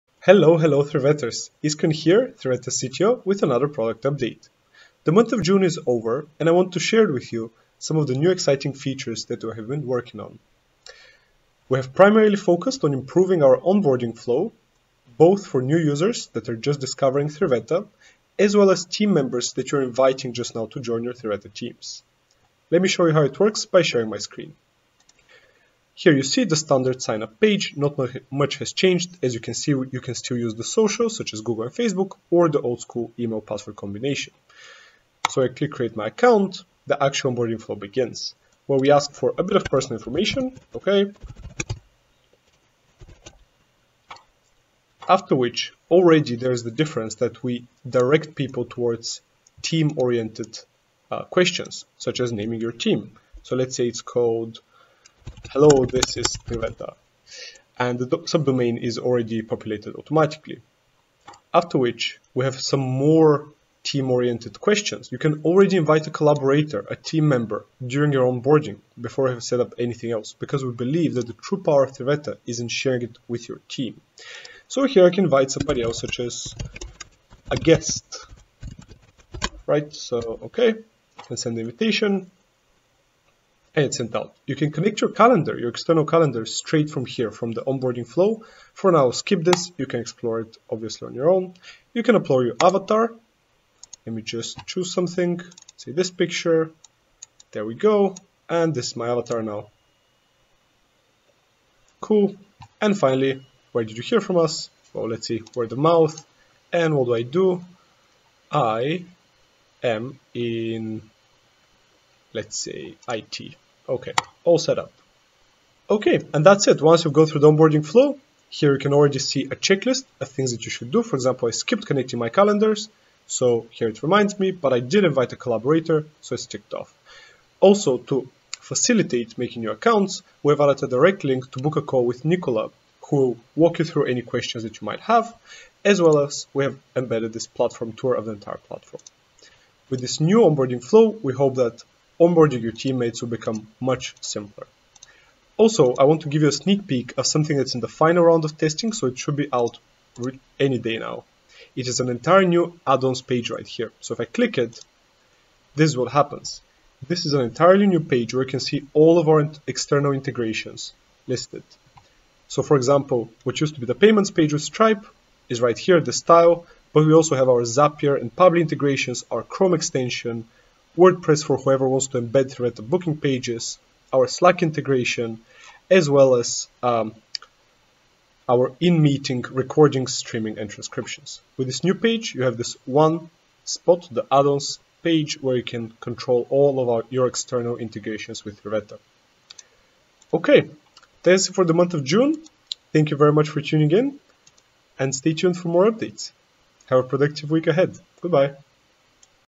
Product Update Round-Up June 2022 | 3veta Product Update
We are constantly improving 3veta. Here's what's new in June 2022:
1. New onboarding flow for new sign-ups
2. Improved onboarding flow for team collaborators
3. Add-ons page which shows all 3veta integrations (Chrome Extension, Slack integration, Audio Transcriptions, Cloud recordings, etc.)